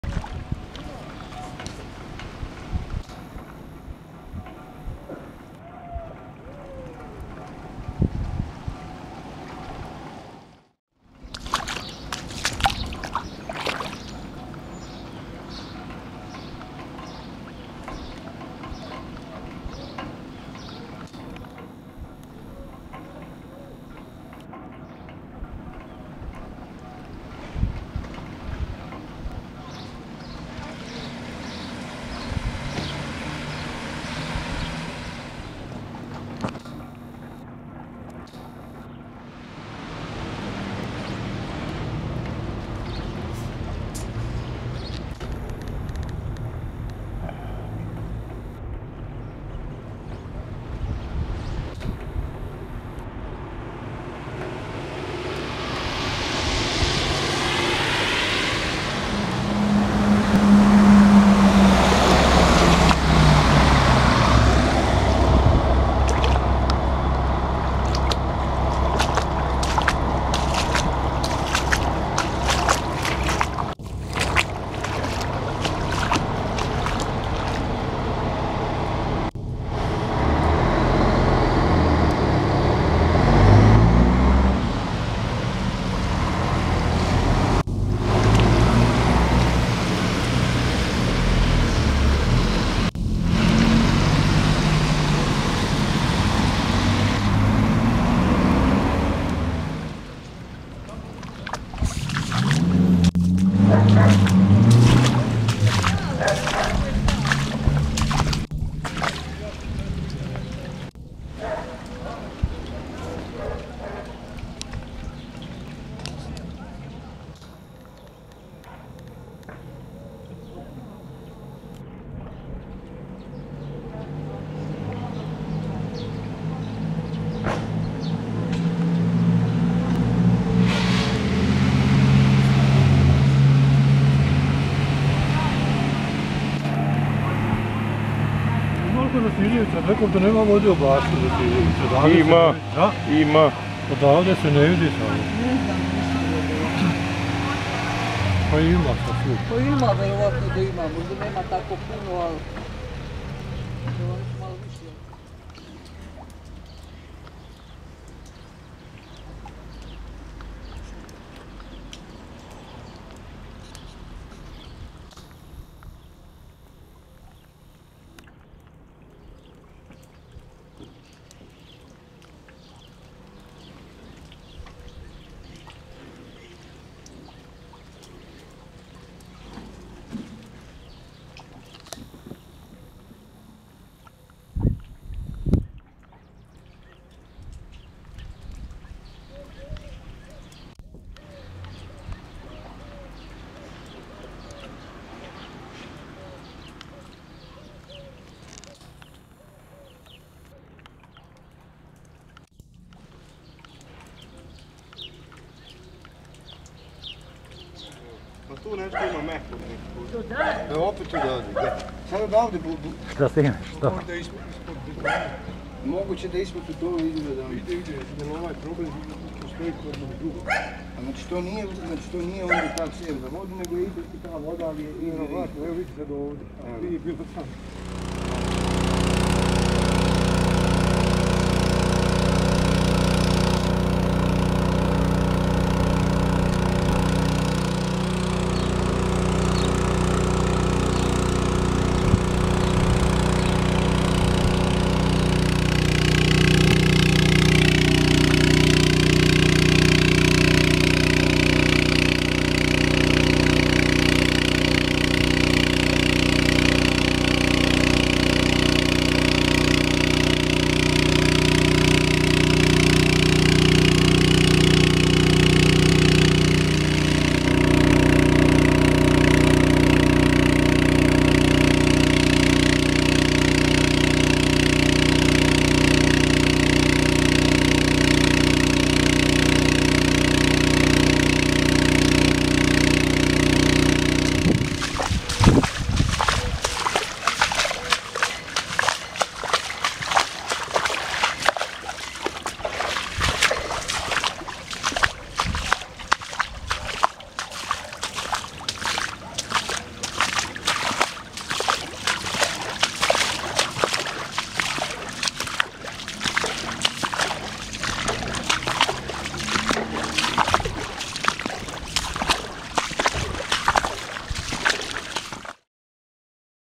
Видовдан у Српској Црњи
Више од 12. сати  падала је киша! Велика  количина  падавина  проузроковала  је  доста  проблема !